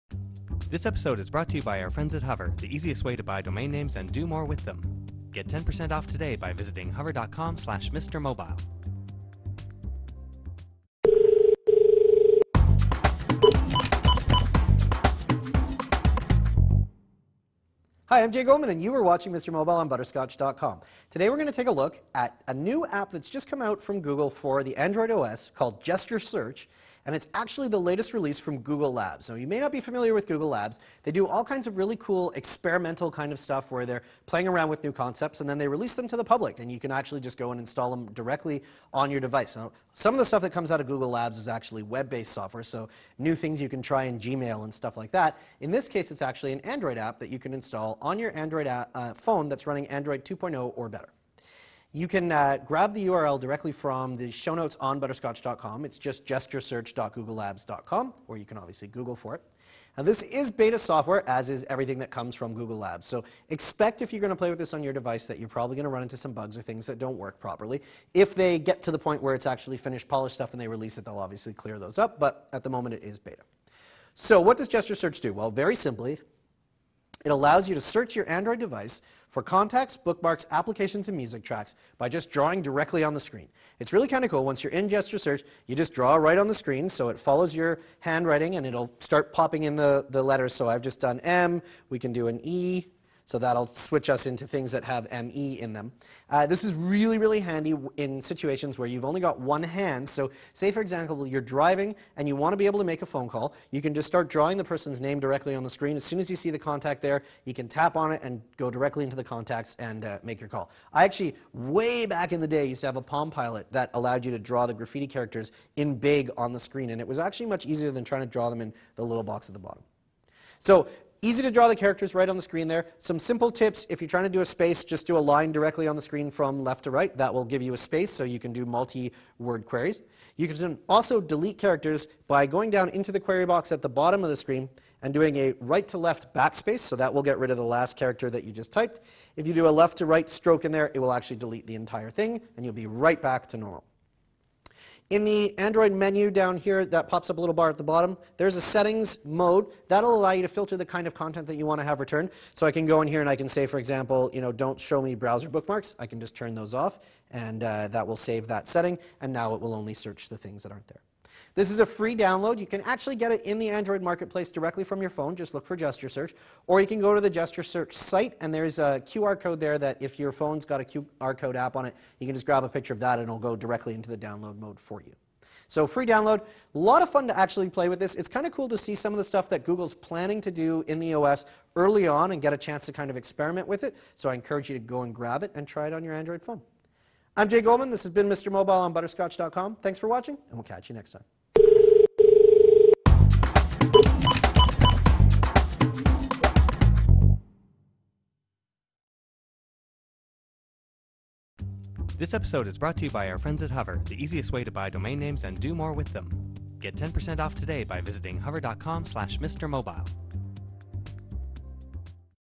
Google Labs' Gesture Search
Google Labs isn't just for Gmail. Jay Goldman takes a look at the awesome free Android app Gesture Search. With this handy app, you can easily weed through all the content on your Android phone @ butterscotch.com